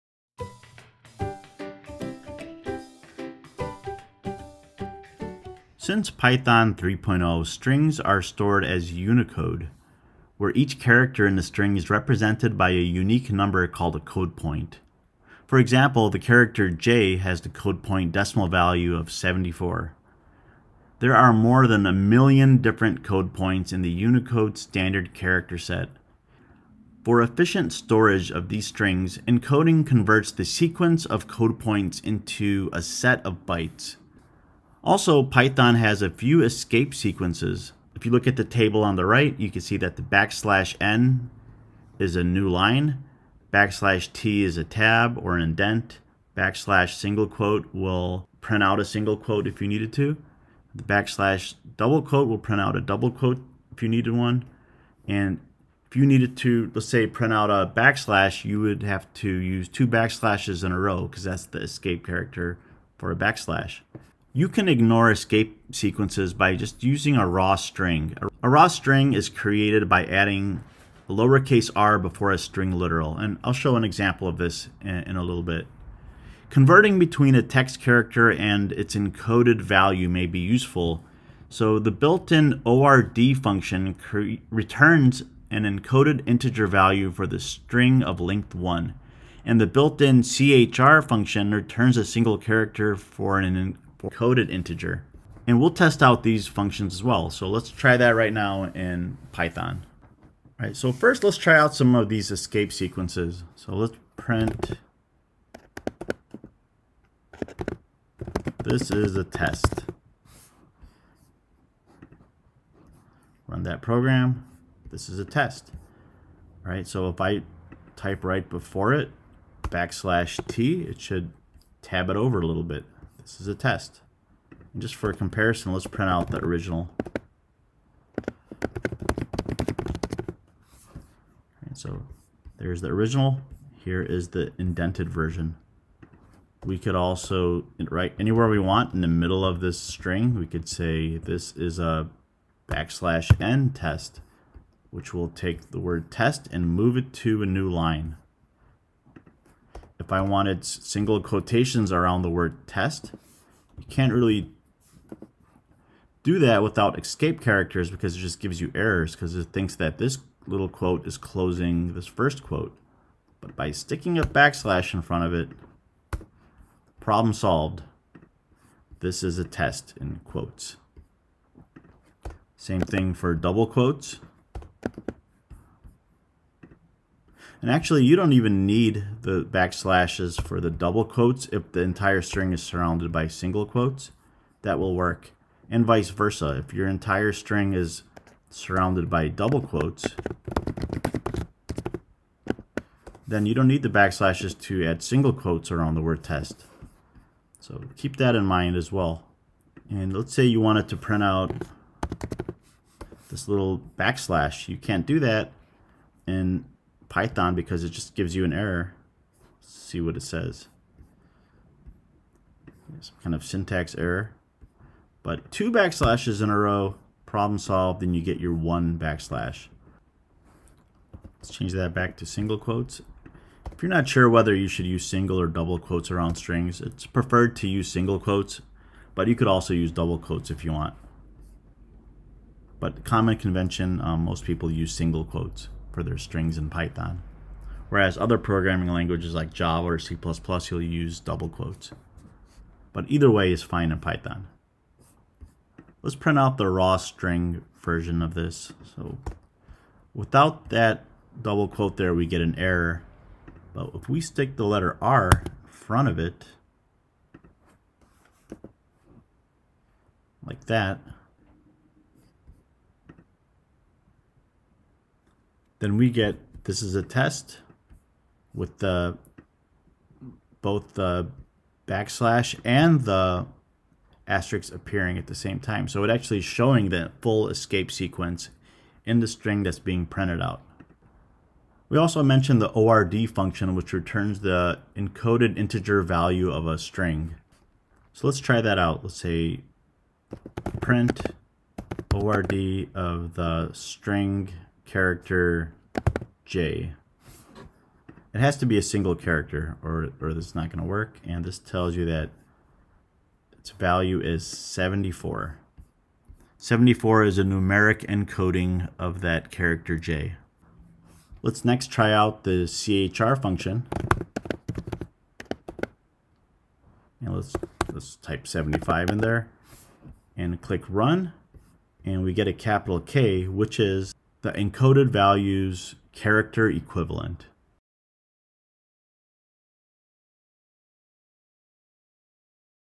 Python Escape Sequences, Text, and Unicode Characters
Since Python 3.0, strings are stored as Unicode, where each character in the string is represented by a unique number called a code point. For example, the character ‘J' has the code point decimal value of 74.  There is more than 1 million code points in the Unicode standard character set. For efficient storage of these strings, encoding converts the sequence of code points into a set of bytes.

Python has the following escape sequences
\n - new line
\t – tab or indent
\’ – single quote
\” - double quote
\\ - backslash

You can ignore escape sequences by using a raw string. A raw string is created by adding an 'r' before a string literal, as in r'this is a test\'', which would output as this is a test\’.

Converting between a text character and it’s encoded value may be useful. The built-in ord() function returns an encoded integer value for a string of length one. The built-in chr() function returns a single character string for an encoded integer.

Ex: chr(75) returns K
Ex: ord(‘B’) returns 66